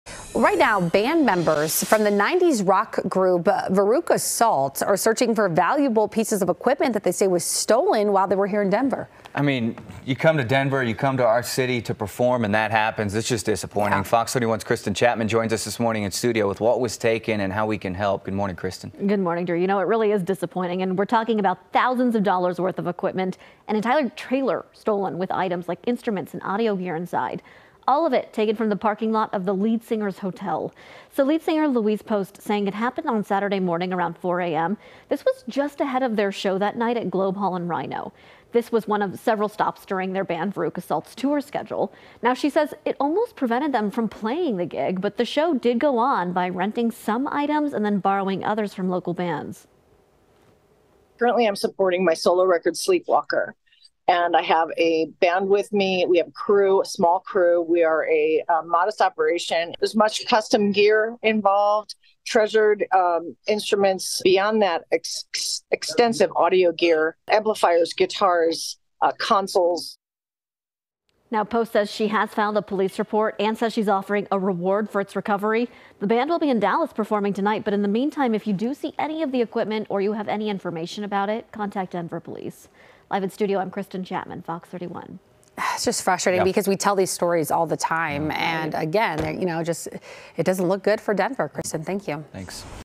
Veruca Salt lead singer says trailer full of equipment stolen in Denver
Louise Post, the lead singer of the rock band Veruca Salt told FOX31 a trailer containing instruments, audio equipment and other gear necessary for a concert was stolen from a hotel parking lot early Saturday morning. Kristen Chapman reports.